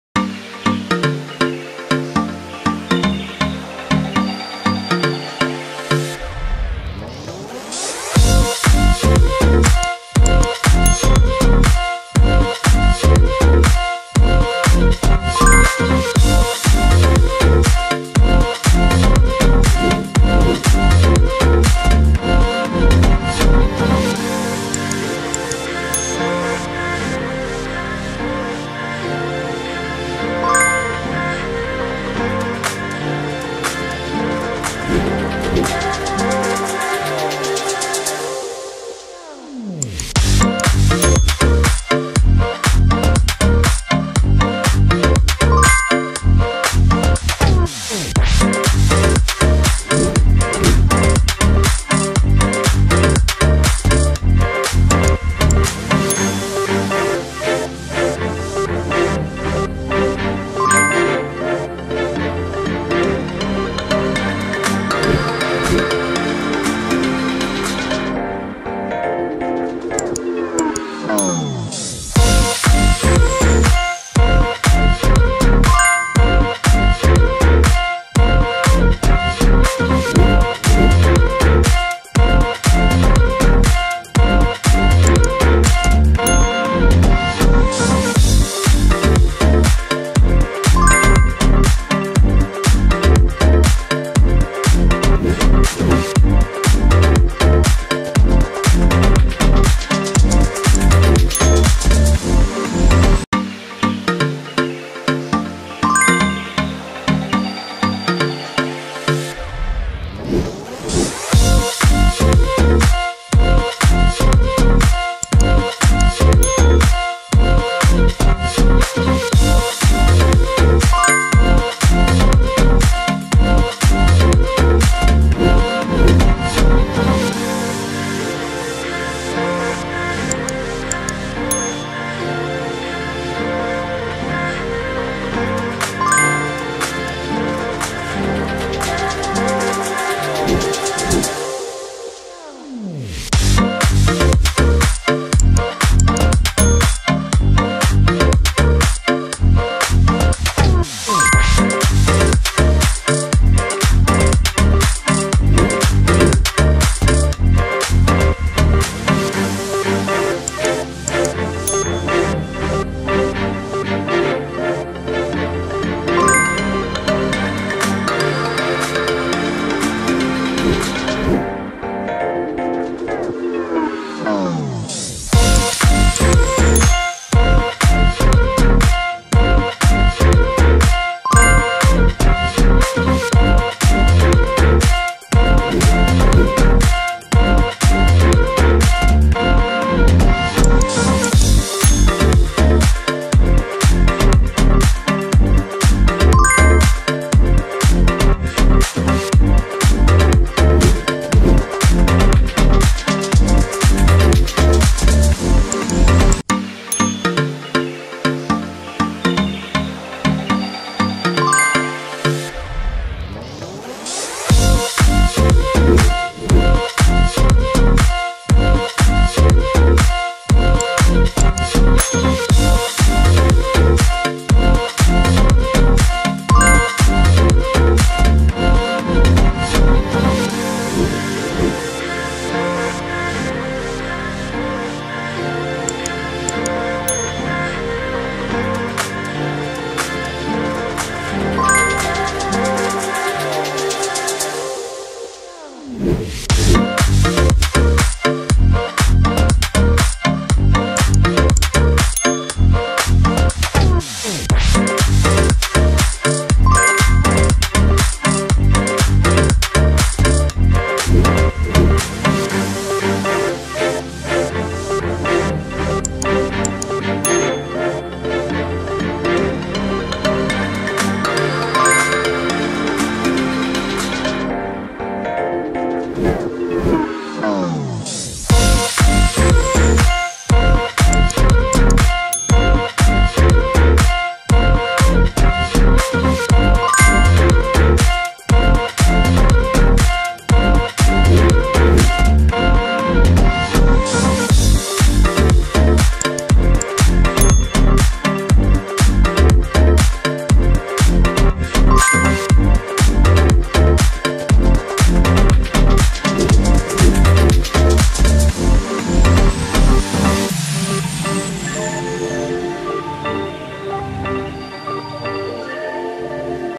Only A Genius Can Guess These Flipped Words! 🔄🧩
Think you’re a genius? Only A Genius Can Guess These Flipped Words! 🔄🧩 Test your brain with this fun and challenging quiz where words are flipped upside down or reversed. Can you figure out the correct words?

Challenge yourself and see how many you can guess right. Share with friends and family to see who’s the real genius!